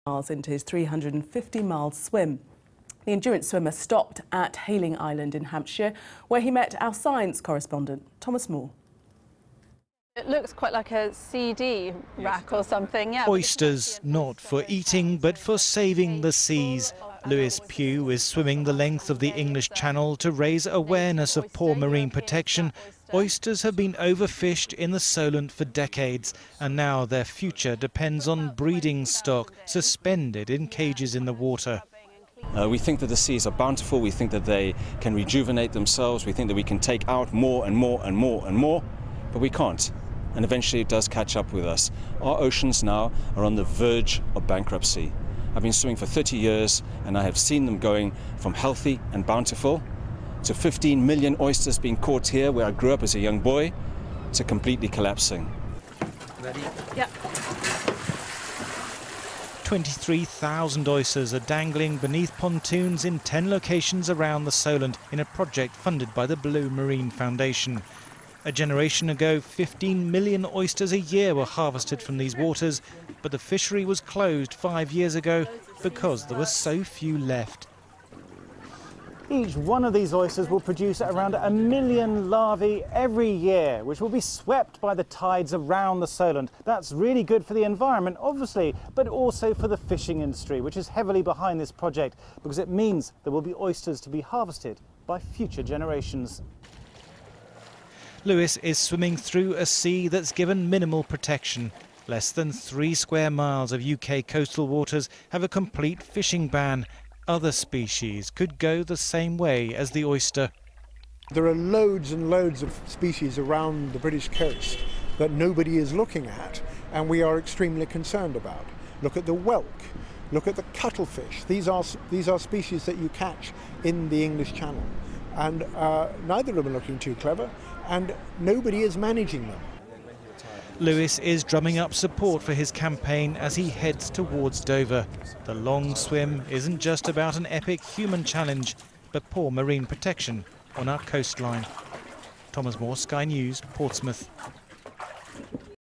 Oysters finally reproducing after decades of being overfished
Lewis Pugh's objective on his Long Swim was always to highlight the damage that humans are doing to Oceans and today he found a prime example of people trying to do their bit to revive them.

Oysters had all but disappeared from Portsmouth - largely because of over-fishing - but now they are back.

Researchers reintroduced thousands of adult molluscs last year and they are now reproducing on their own.

Lewis is now almost 229 miles into his 350 miles swim.  The endurance swimmer stopped at Hayling Island in Hampshire where he met our Science Correspondent Thomas Moore.

and download our apps